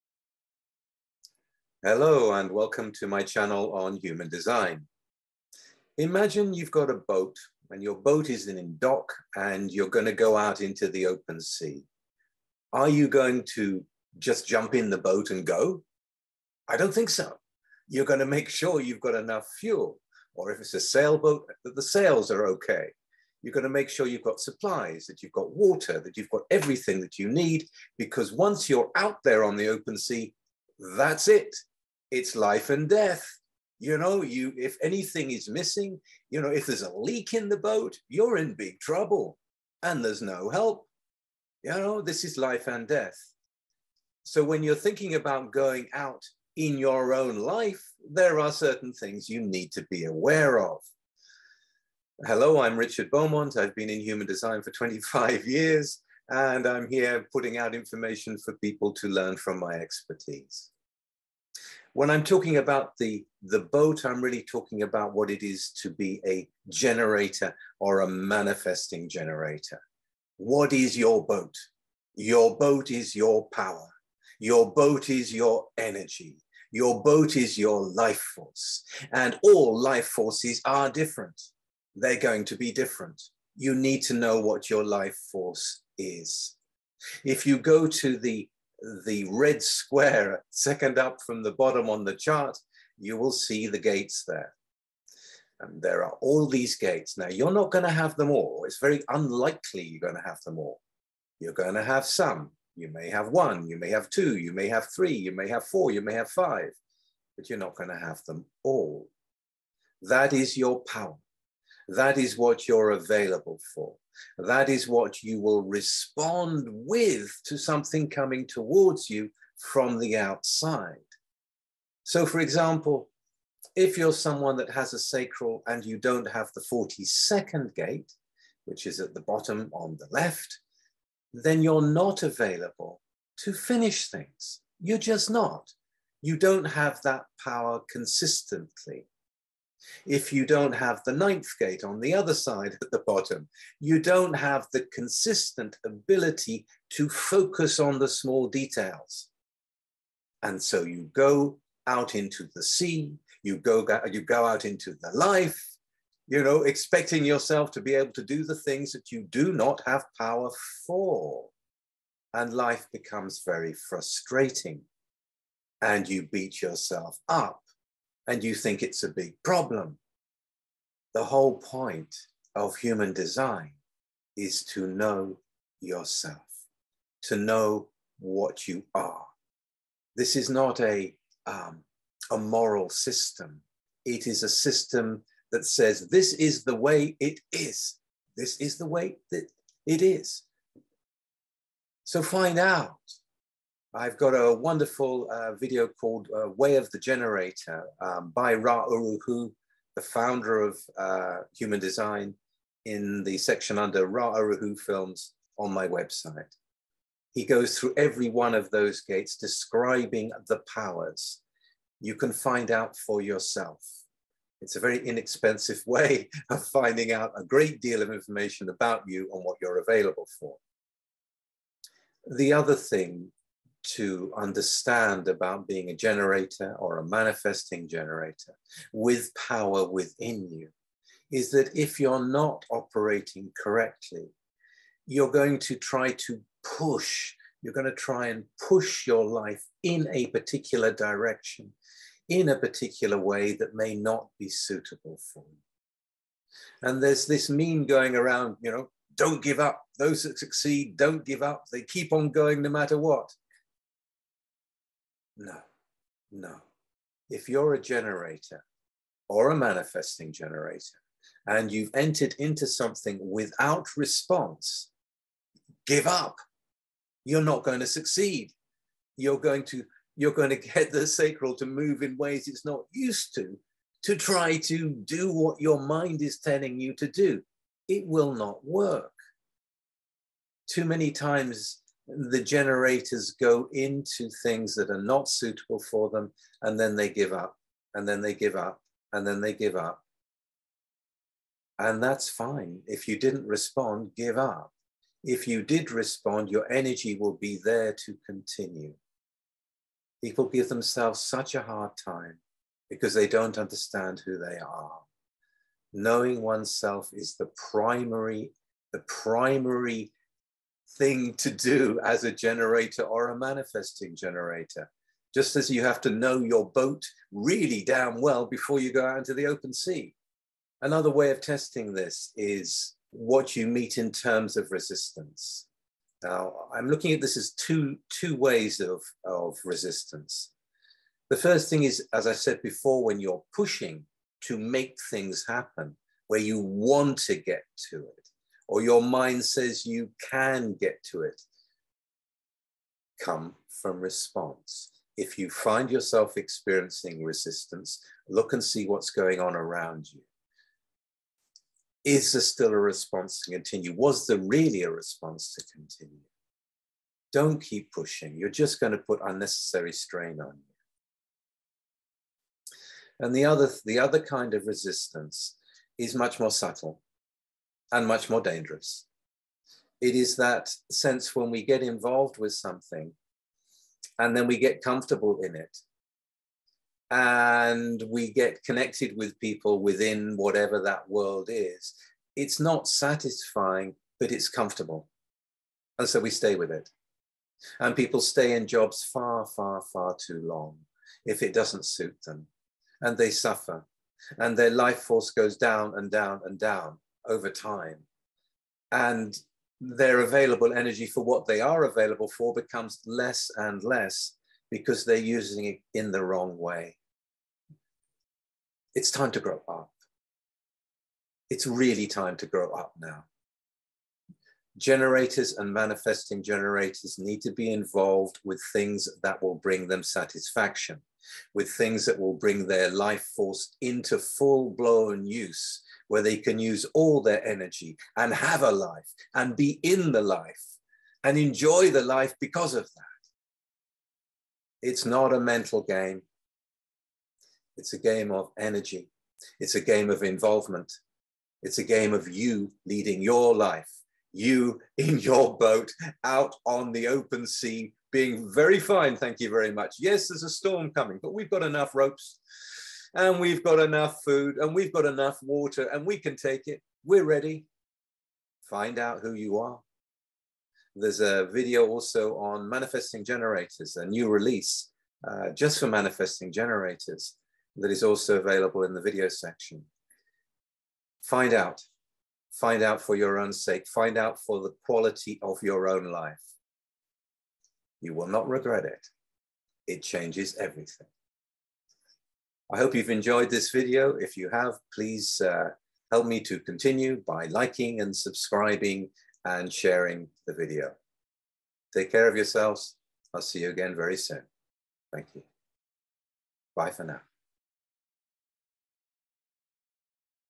What You Need To Know As A Manifesting Generator
Richard Beaumont zeros his focus in on the essential considerations for every Manifesting Generator and Pure Generator.